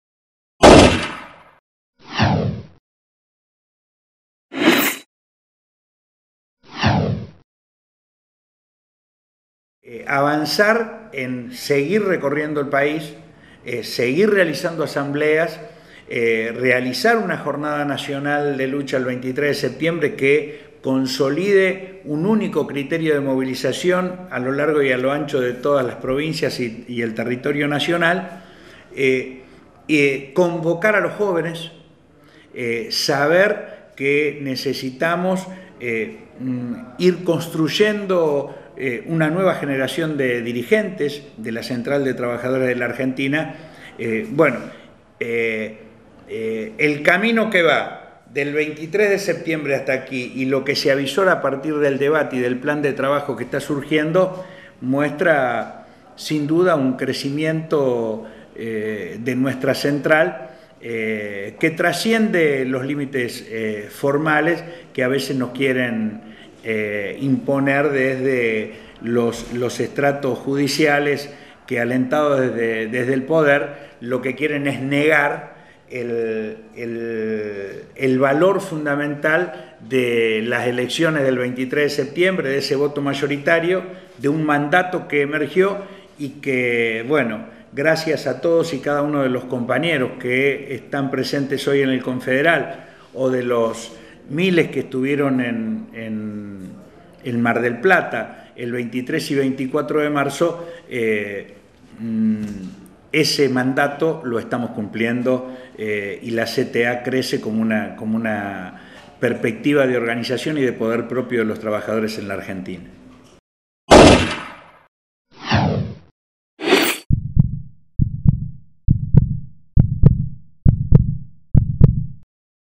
Hugo Godoy - "Hay que convocar a los jóvenes"- Secretario General Adjunto ATE Nacional
Entrevista a Hugo "Cachorro" Godoy, secretario general adjunto de ATE Nacional, durante el 2do. Consejo Federal de la Central de Trabajadores de la Argentina el 17 de agosto de 2011.